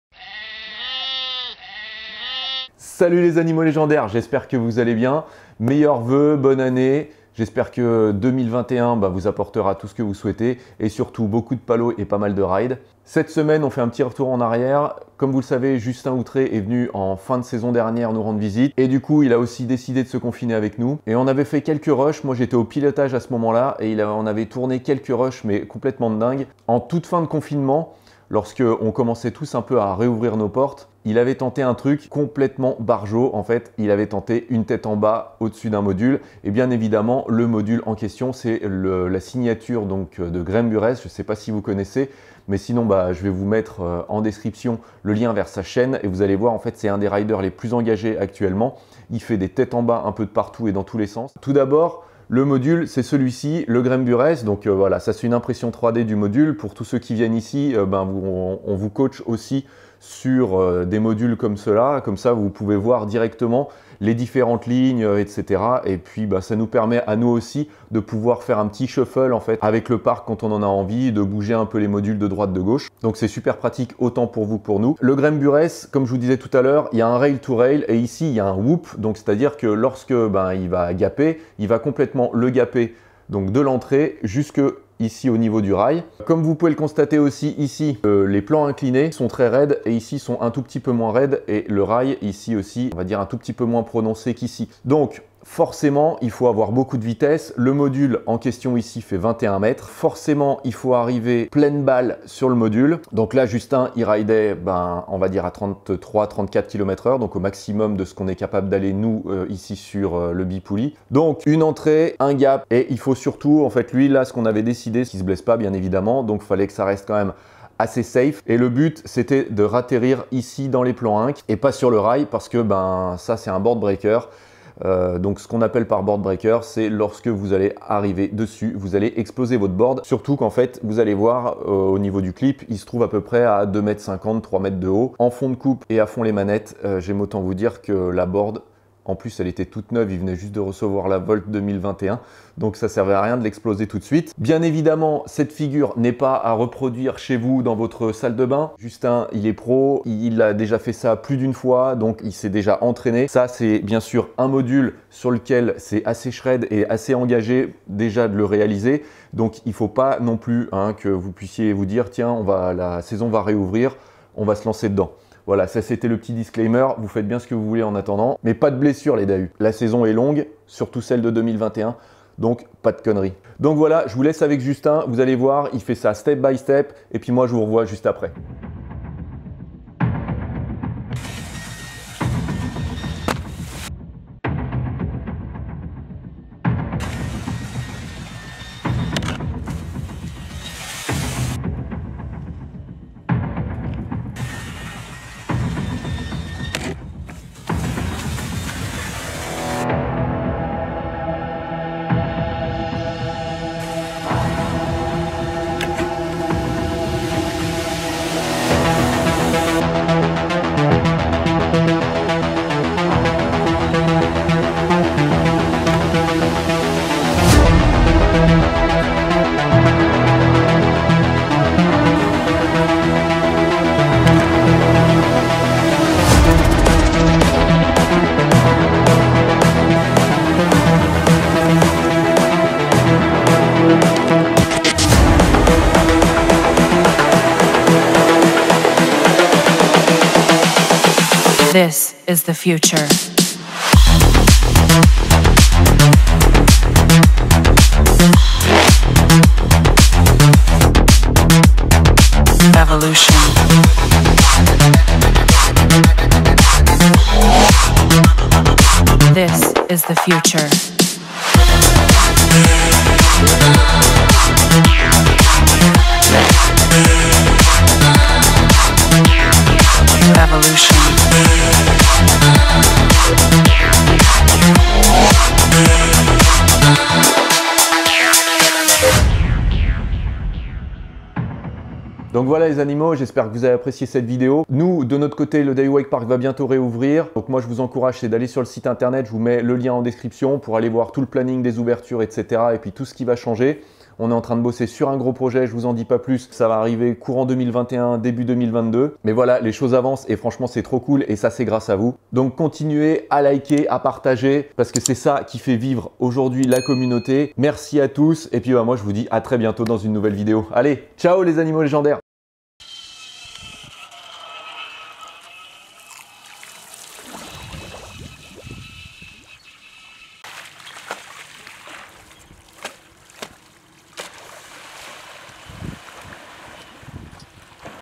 [RIDE] Justin se retourne sur le GRAEME !
En fin d'année 2020, Justin se retourne la tête sur le plus gros module du Park !
Si vous êtes passionné(e)s de sports extrêmes et que vous tombez sur cette vidéo, partagez-là à un pote qui est tête brûlé 😉 On attends la saison 2021, pour savoir jusqu'au Justin va pouvoir aller ? en blind ? en tantrum ? avec le rail derrière ?